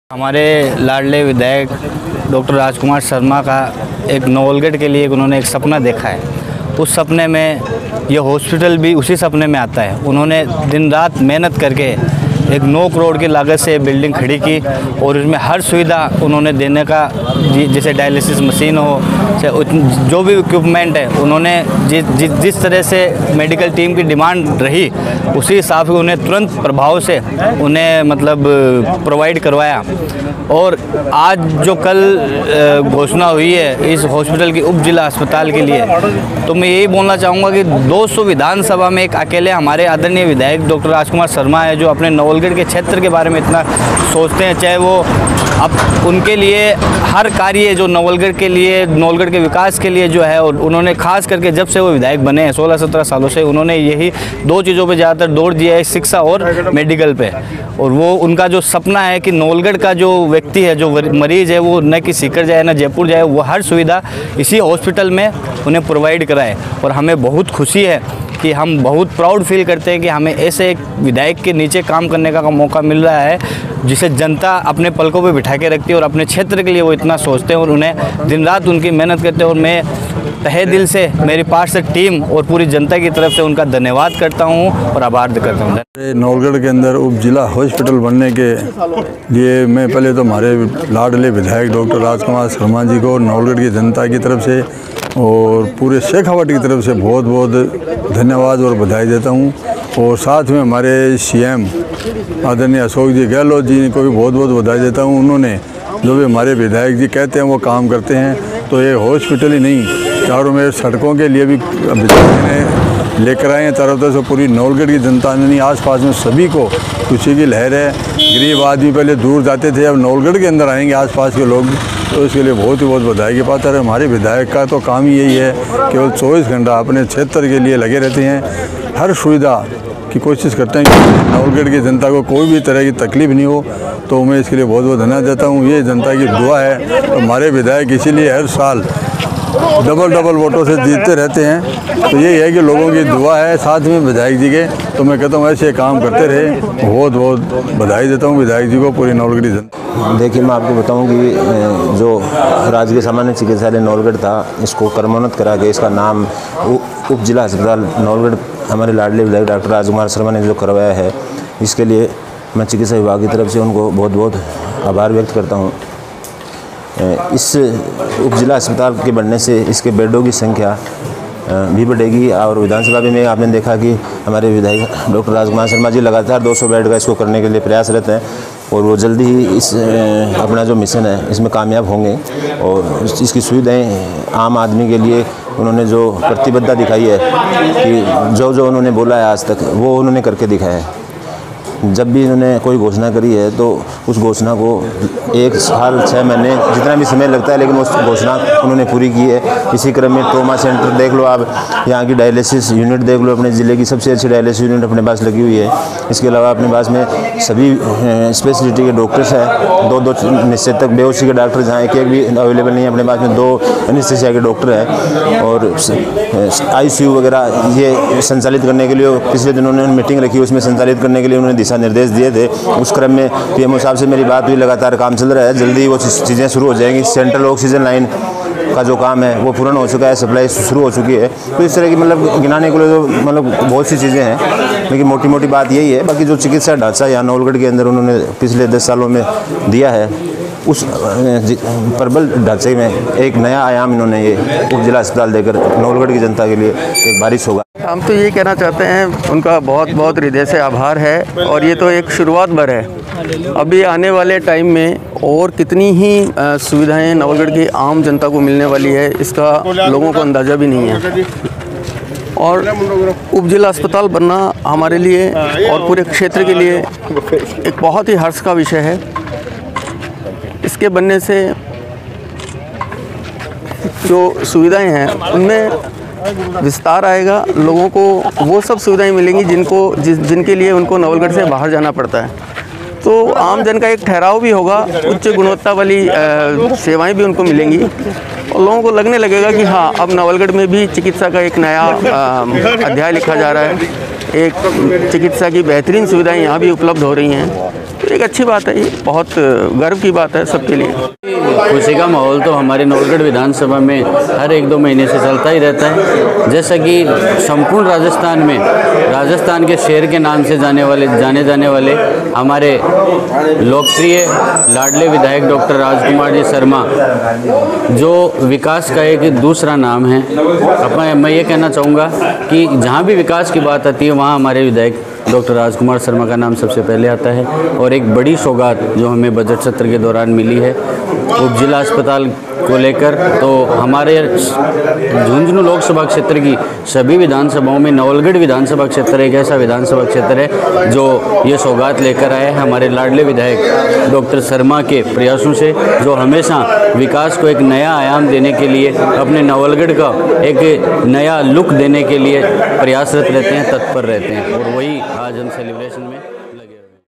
नवलगढ़ राजकीय अस्पताल को उप अस्पताल तक दर्जा मिलने पर मनाई खुशी, डॉ. राजकुमार शर्मा का जताया आभार...
नवलगढ़ राजकीय अस्पताल को उप अस्पताल तक दर्जा मिलने पर मनाई खुशी, विधायक डॉ. राजकुमार शर्मा का जताया आभार...